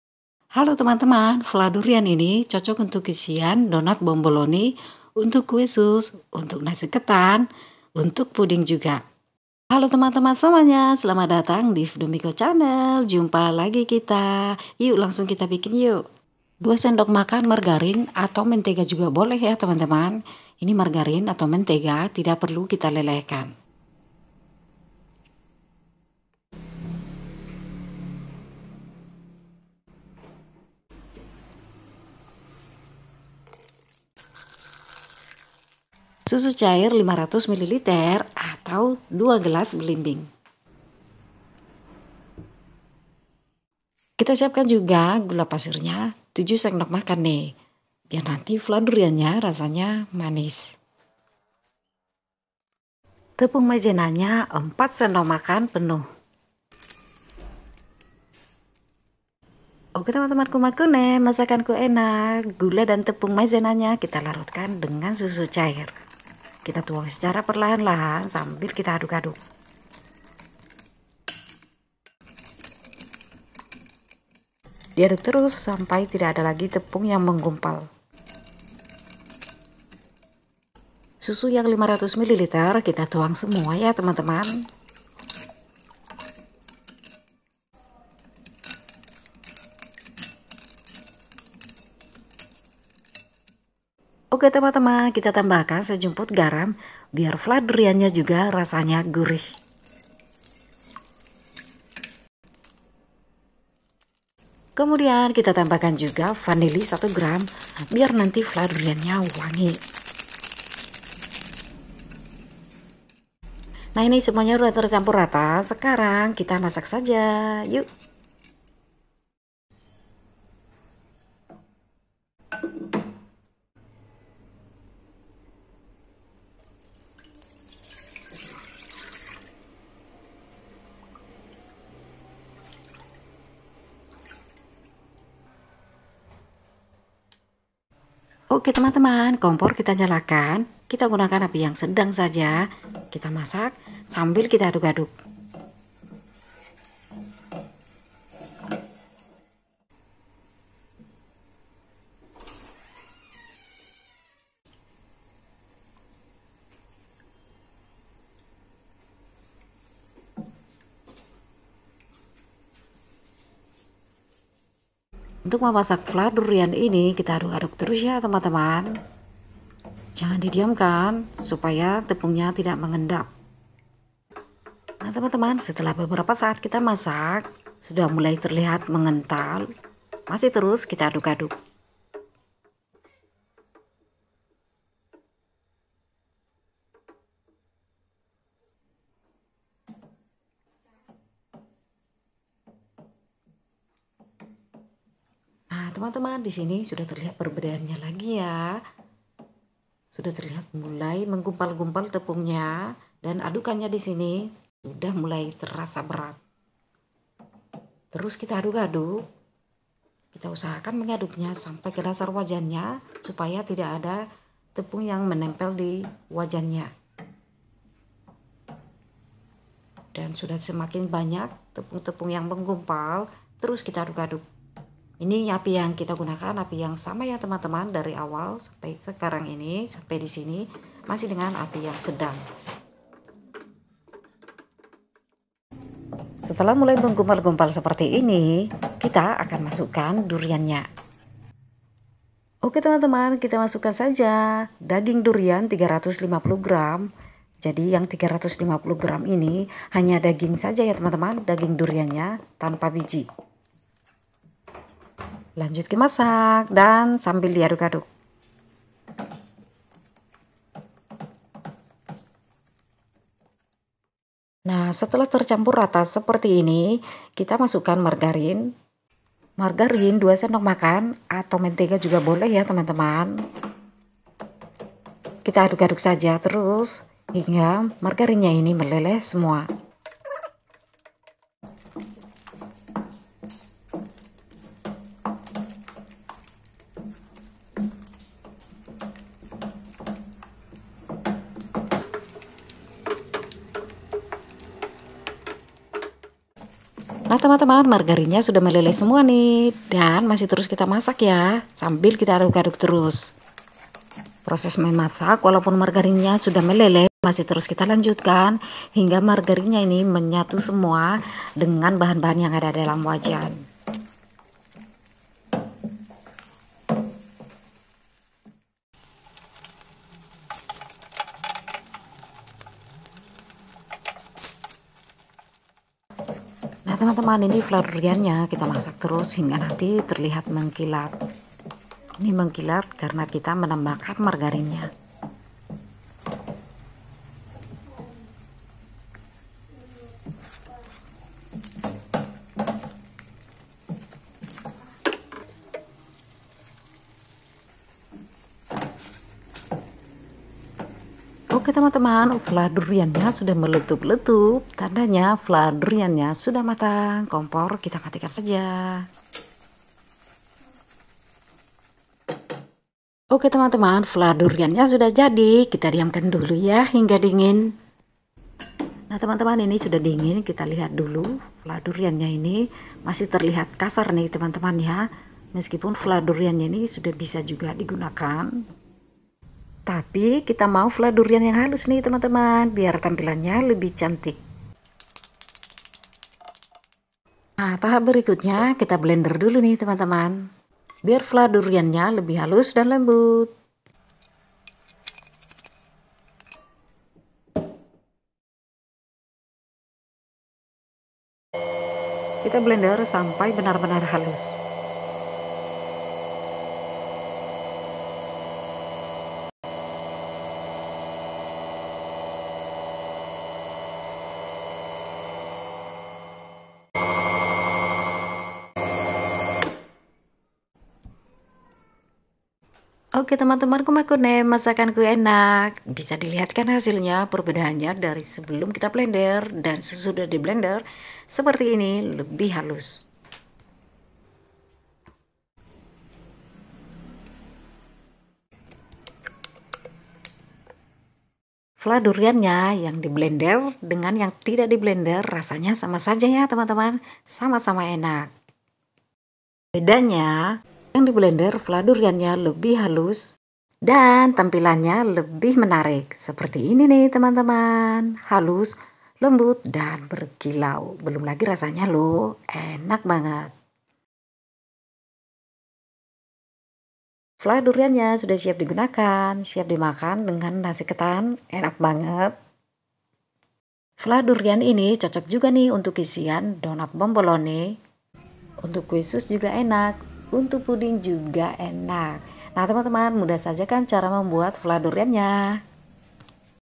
Cara membuat vla durian enak lembut
Vla durian enak dan lembut untuk isian donat bomboloni, untuk kue sus, untuk ketan ,untuk puding dsb.

Resep dan bahan dan cara membuat  membuat  vla durian sangat mudah.
Bahan:
Durian tanpa biji 350grm
Margarin/mentega 2sdm
Susu cair 500ml
Gula pasir 7sdm
Vanilli 1grm
Tepung maizena 4sdm

Video Menu olahan sederhana enak lainnya

Terima kasih.